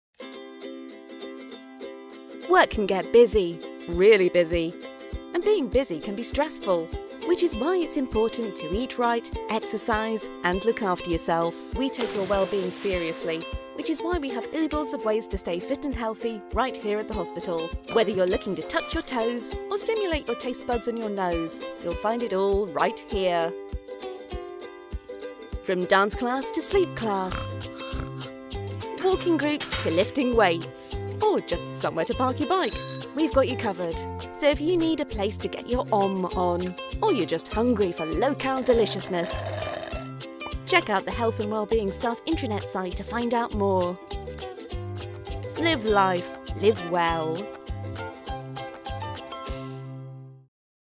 Live Life Live Well
There are lots of ways you can maintain and improve your health and well-being if you work at Northampton General Hospital.